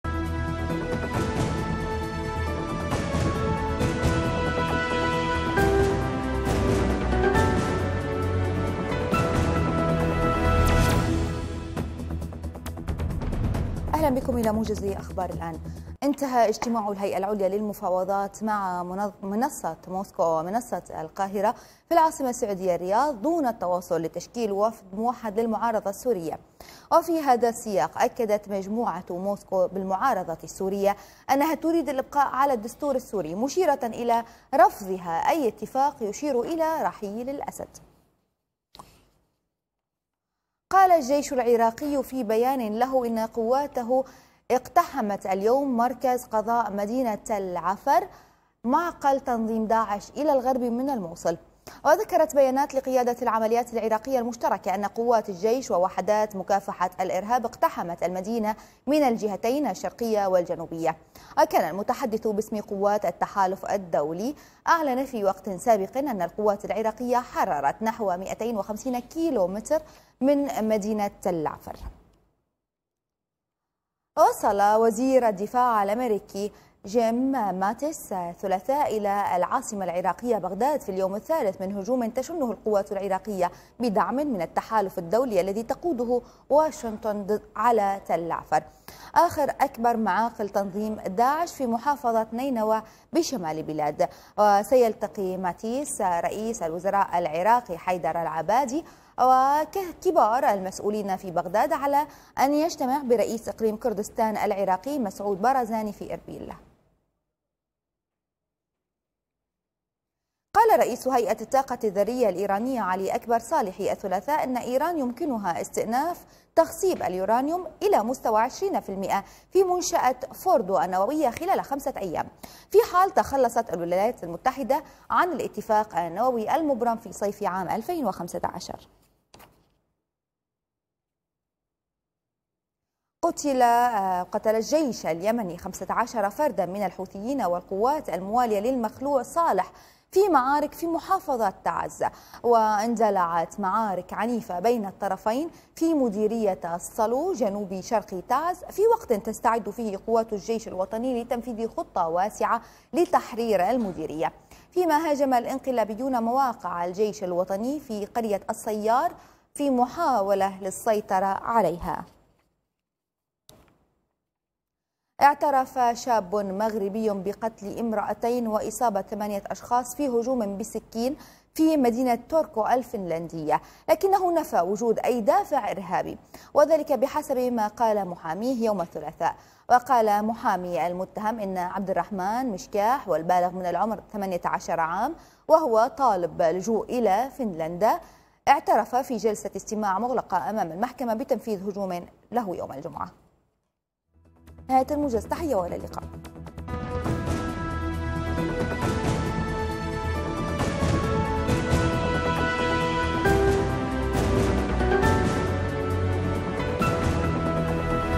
22/8/2017 | موجز الخامسة عصراً لأهم الأخبار من
العناوين:
-اتفاق جزئي بين وفدي الهيئة العليا ومجموعة القاهرة في المعارضة السورية
- غارات جوية على داعش بالرقة
- الجيش العراقي يواصل تقدمه في
- وزير الدفاع الأمريكي يصل إلى بغداد في اليوم الثالث من هجوم على آخر أكبر معاقل
- ايران يمكنها استئناف تخصيب اليورانيوم بستوى 20% في خمسة أيام
- منفذ الهجوم بالسكين في فنلندا يعترف بجريمته

تقديم: شيرين الرفاعي